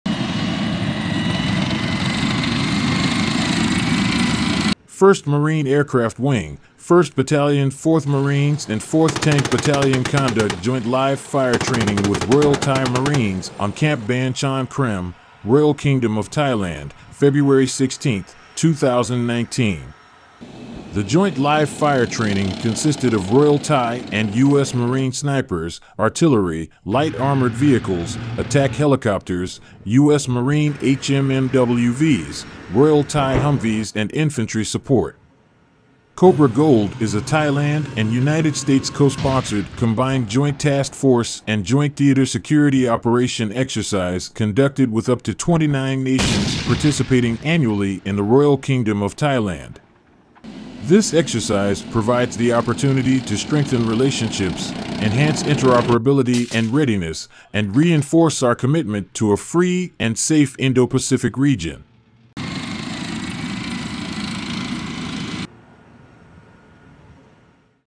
U.S. Marine and Royal Thai live-fire joint training
1st Marine Aircraft Wing, 1st Battalion 4th Marines and 4th Tank Battalion conduct joint live fire training with Royal Thai Marines on Camp Ban Chan Krem, Royal Kingdom of Thailand, Feb. 16, 2019. The joint live fire training consisted of Royal Thai and U.S. Marine snipers, artillery, light armored vehicles, attack helicopters, U.S. Marine HMMWVs, Royal Thai humvees and infantry support. Cobra Gold is a Thailand and United States co-sponsored Combined Joint Task Force and joint theatre security operation exercise conducted with up to 29 nations participating annually in the Royal Kingdom of Thailand. This exercise provides the opportunity to strengthen relationships, enhance interoperability and readiness, and reinforce our commitment to a free and safe Indo-Pacific region.

Video by [U.S. Marine Corps photo by Lance Cpl. Ryan Persinger]